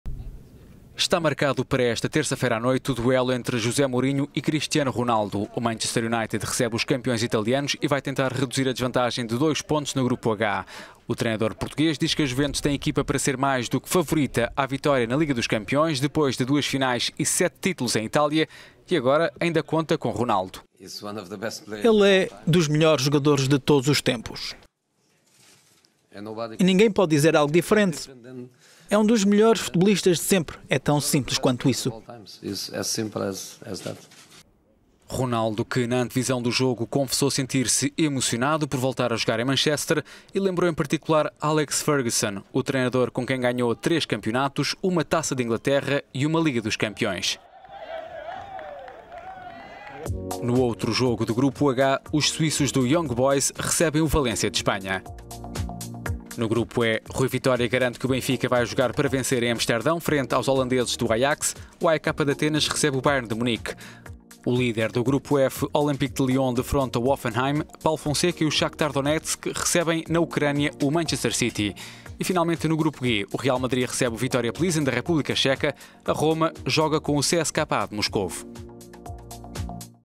Liga dos Campeões: Duelo Mourinho - Ronaldo
Manchester United recebe esta terça-feira a Juventus na terceira jornada da Liga dos Campeões. Chileno Alexis Sanchez não joga pelos diabos vermelhos.…